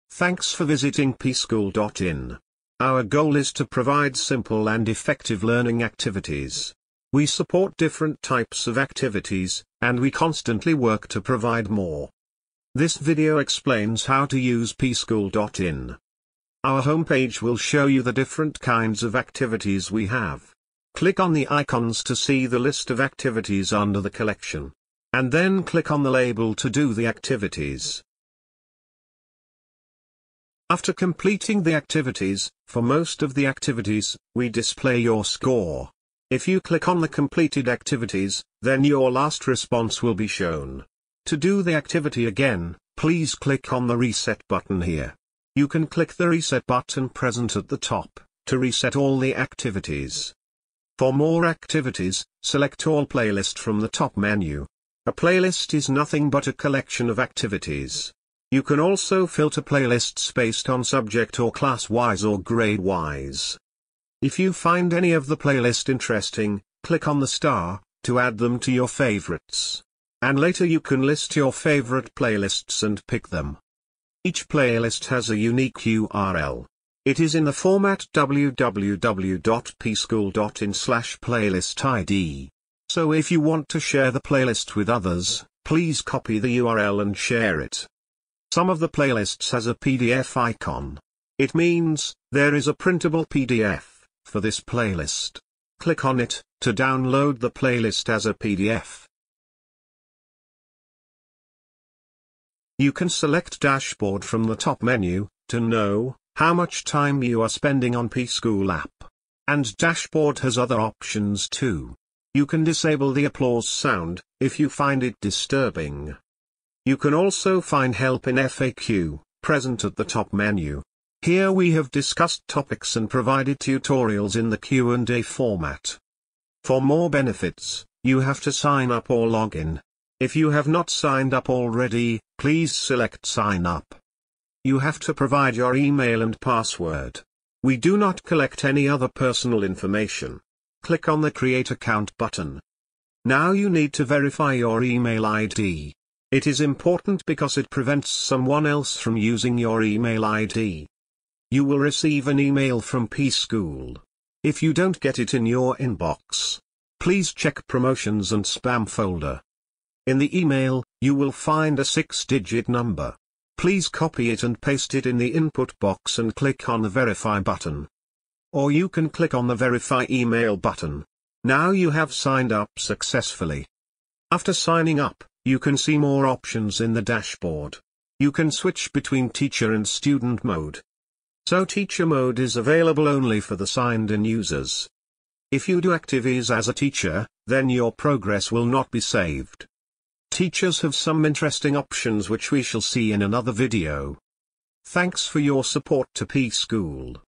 Getting started with PSchool
PSchool is basically an activity based learning app for kids. We cover different topics like coloring, writing, math, grammar, vocabulary, clock, science, sudoku, crossword, wordsearch, etc.

We also provide authoring tools, so that anybody can create activities like MCQ, drag and drop, fill up etc, and they can also print them in A4 sheet.

Teachers can create classrooms, give the activities as homework and track the students response.

Schools, bloggers can embed Pschool inside their app with their own branding.

This video is part of a series that explains the features of Pschool app:

All videos